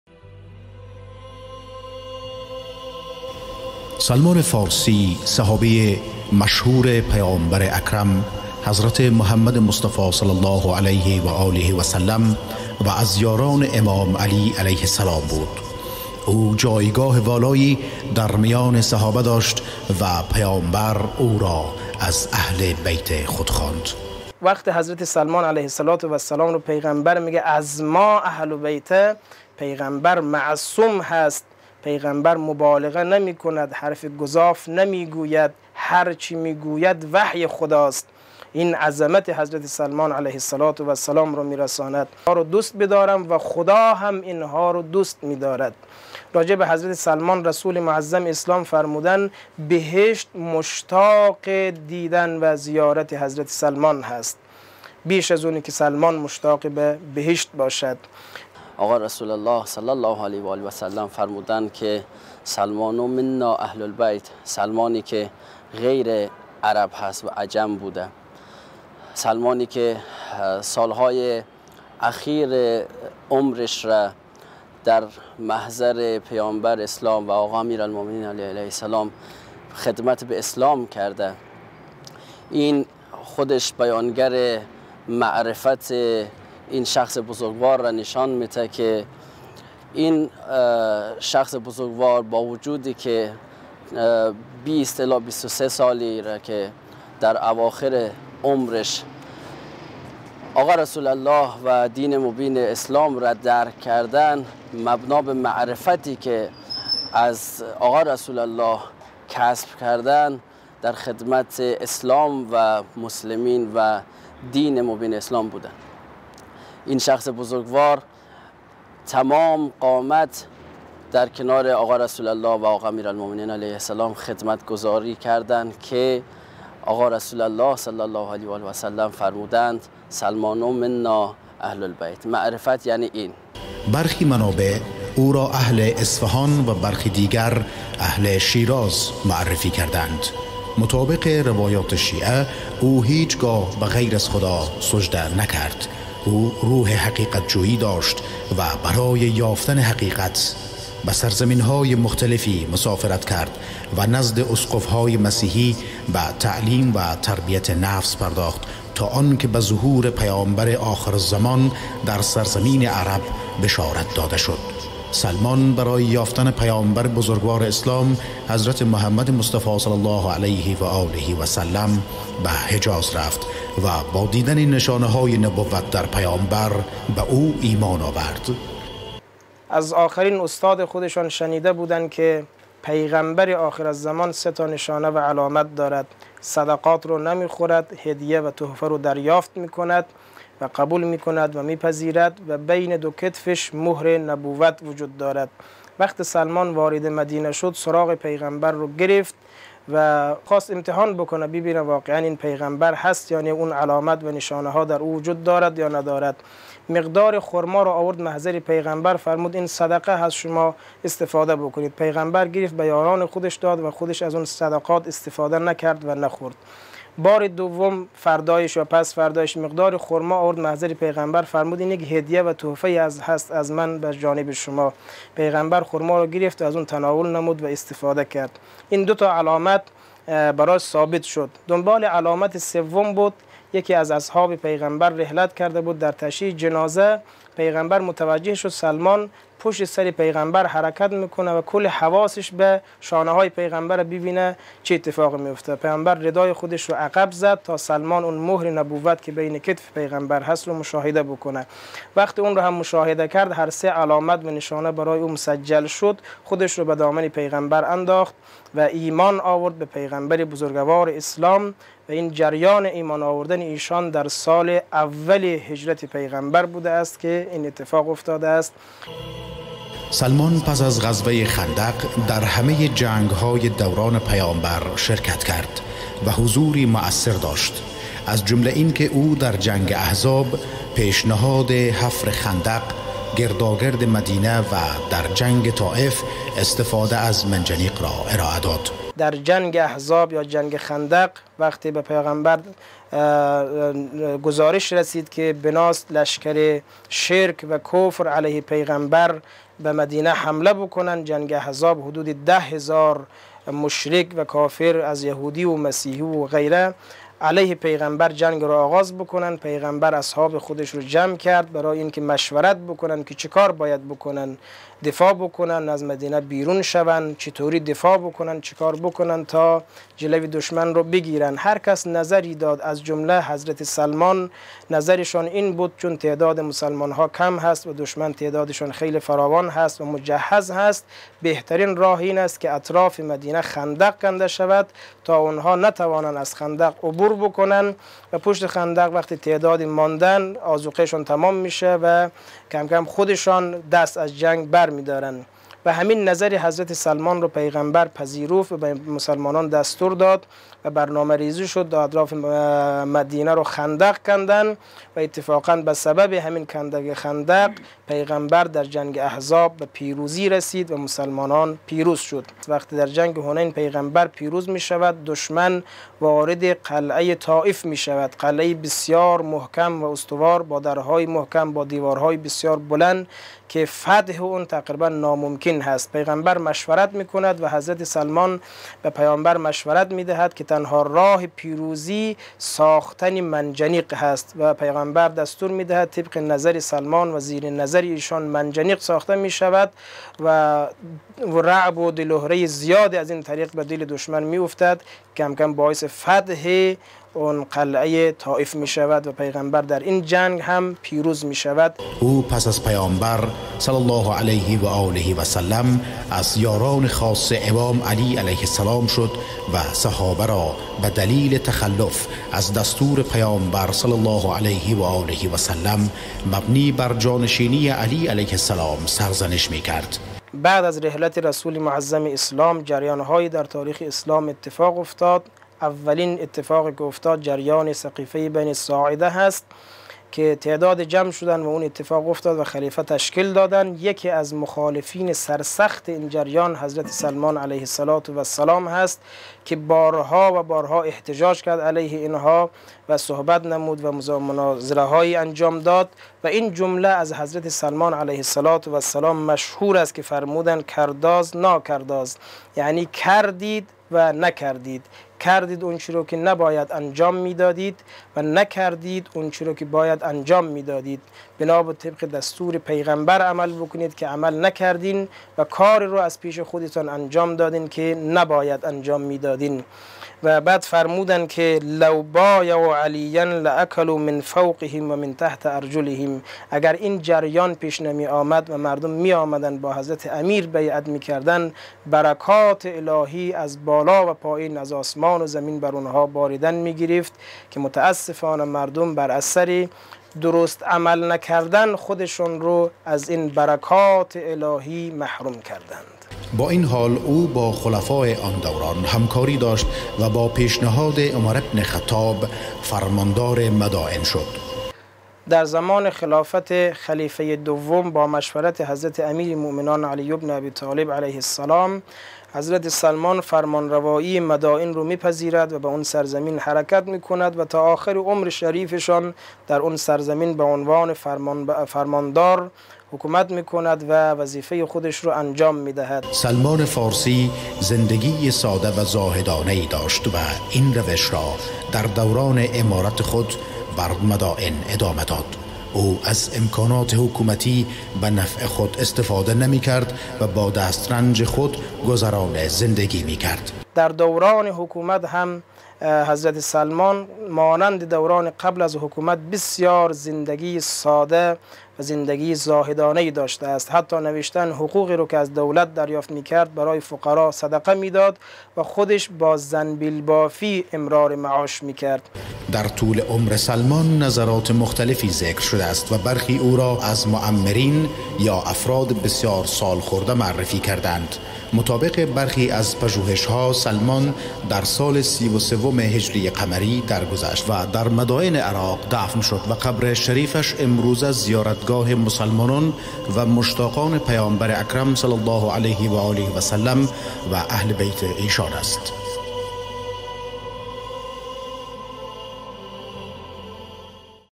مستند گزارشی وفات سلمان فارسی(ع) / ۸ صفر
موضوع: زندگی‌نامه سلمان فارسی(ع)
راوی/نریتور: محمد جاوید